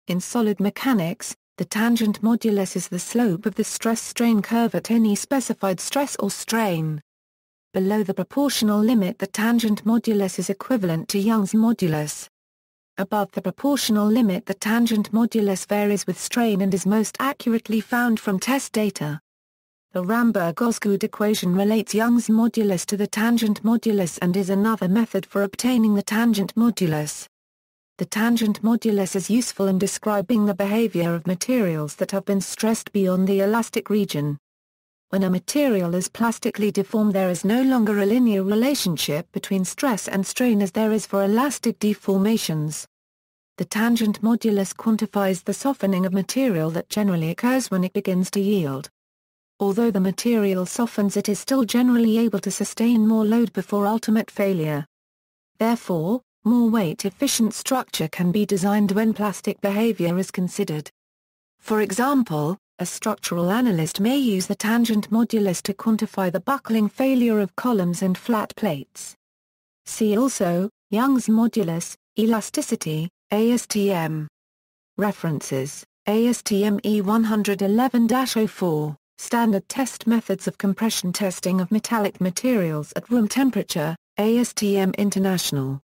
Tangent modulus
In solid mechanics, the tangent modulus is the slope of the stress-strain curve at any specified stress or strain. Below the proportional limit the tangent modulus is equivalent to Young's modulus. Above the proportional limit the tangent modulus varies with strain and is most accurately found from test data. The Ramberg-Osgood equation relates Young's modulus to the tangent modulus and is another method for obtaining the tangent modulus.
The tangent modulus is useful in describing the behavior of materials that have been stressed beyond the elastic region. When a material is plastically deformed there is no longer a linear relationship between stress and strain as there is for elastic deformations. The tangent modulus quantifies the "softening" of material that generally occurs when it begins to yield.